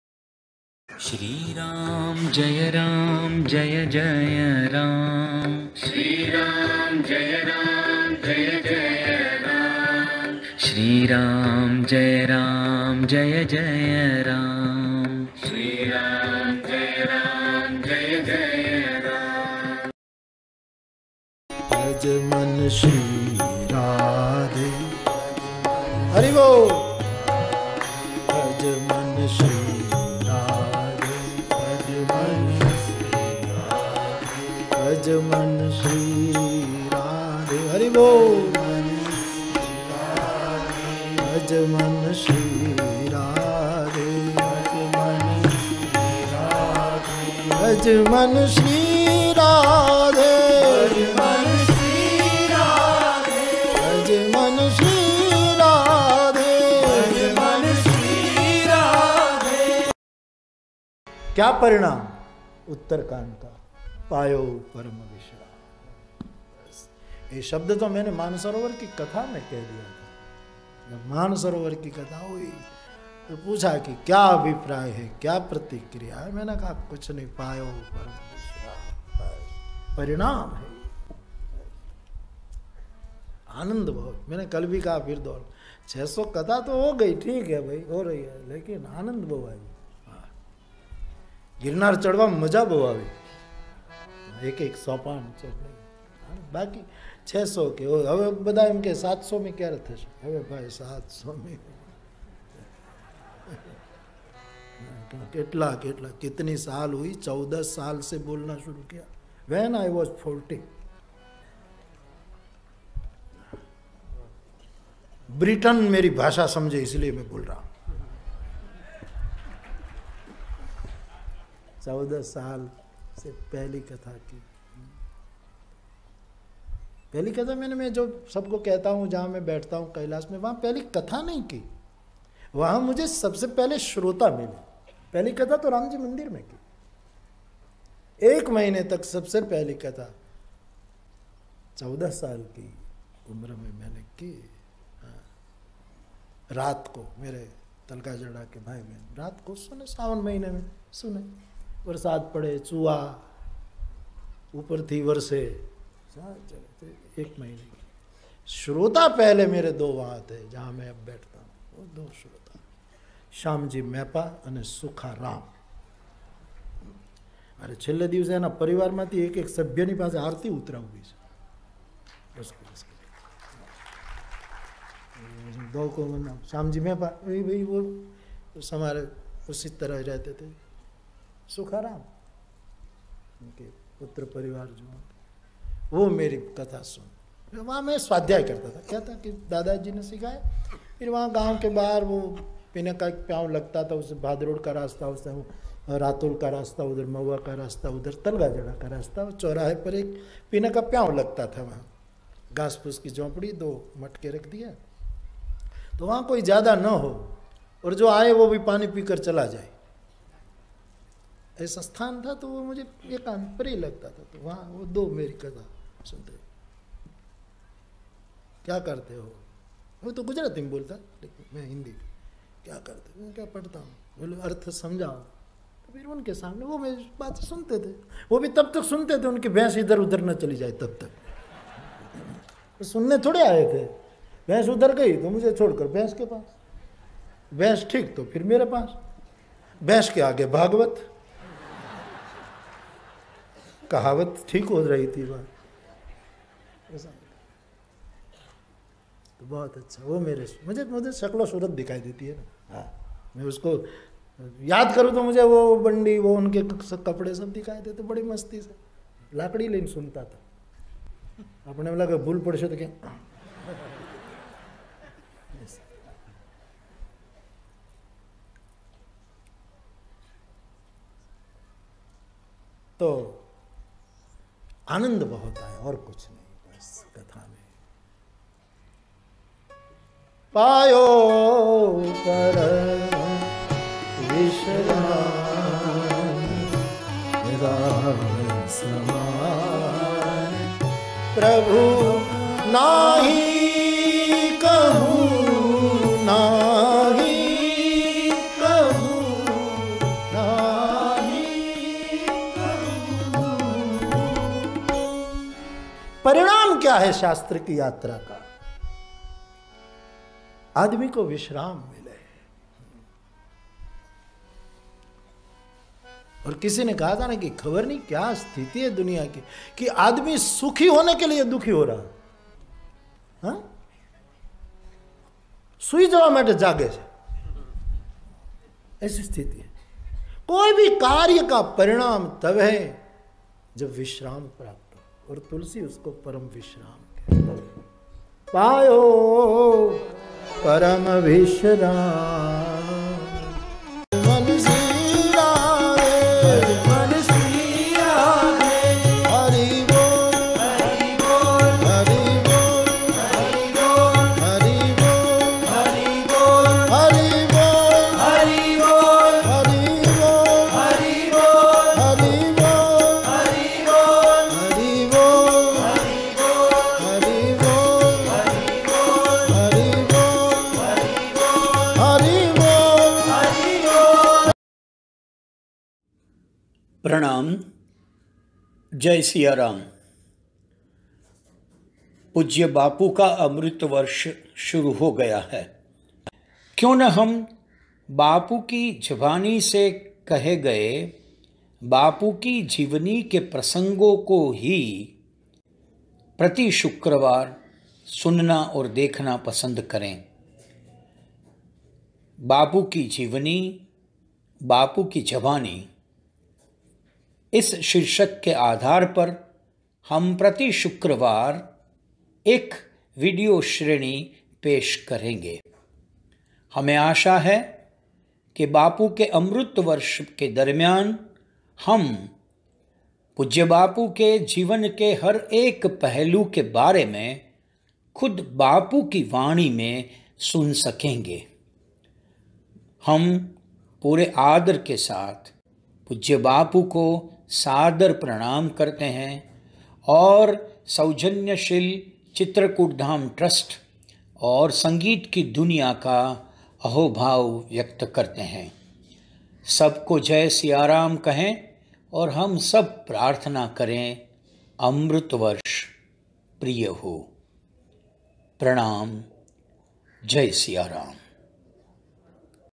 Bhadrayu || बापुकी जीवनी, बापुकी जबानी || (०५) : प्रति शुक्रवार
::आजका मोती::  " पायो परम विश्राम || "

पूज्य श्री मोरारीबापु के जीवनका एक एक नया कदम ::
अगली महाशिवरात्रि तक ::

|| आओ, बापू की वाणी में बापू को जानें ||
|| अमृत वर्ष प्रिय हो ||

भद्रायु का प्रणाम-जय सियाराम

महाशिवरात्रि २१ फरवरी, २०२०::
पूज्य बापुके पचहत्तर वर्ष प्रवेश के उपलक्ष्य में  एक साप्ताहिक विडिओ श्रेणी "बापुकी जीवनी, बापुकी जबानी ||
पूज्य बापुका जीवन खुल्ली किताब सा है || वर्गशिक्षक से लेकर श्लोकशिक्षक की बापुकी जीवनयात्राके पहेलु पूज्य बापुने अपनी अपनी कथाओंके दौरान खुद बयान किये हैं ||  यही पहलुओंको व्यासपीठप्रेमीओ  तक पहुंचाने के मंगल - उदेश्यसे पूज्य बापुके अनुग्रह से पेश की जानेवाली श्रेणीका original source तो श्री चित्रकुटधाम ट्रस्ट, तलगाजरडा और संगीतनी दुनिया, महुवा हैं || हम पूरे आदरसे उनके सौजन्यके लिए हमारी  कृतज्ञता व्यक्त करते हैं |
पूज्य बापु  के श्री चरणोंमे प्रेम प्रणाम अर्पण करके सबको जय सियाराम कहकर प्रार्थना करते हैं कि ||अमृत वर्ष प्रिय हो || भद्रायु

 Our Special thanks to SHREE CHITRAKUTDHAM TRUST  & SANGEETNI DUNIYA
Copyright :
SHREE CHITRAKUTDHAM TRUST
Bhadrayu Vachhrajani